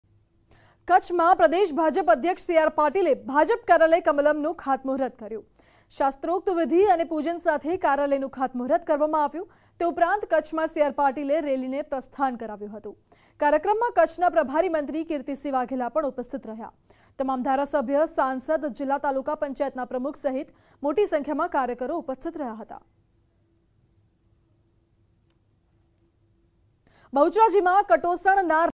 Kutch માં C R Patil એ ભાજપ કાર્યાલય Kamalam નું ખાતમૂહુર્ત કર્યું | BJP | News18 Gujarati
Kutch માં C R Patil એ ભાજપ કાર્યાલય Kamalam નું ખાતમૂહુર્ત કર્યું. શાસ્ત્રોક્ત વિધિ સાથે પૂજન કરીને ખાત મૂહુર્ત કરવામાં આવ્યું. તે ઉપરાંત Kutch માં CR Patil એ Rally ને પ્રસ્થાન કરાવ્યું હતું. આ કાર્યક્રમમાં Kutch ના પ્રભારી મંત્રી Kirtisinh vaghela પણ ઉપસ્થિત રહ્યા હતા.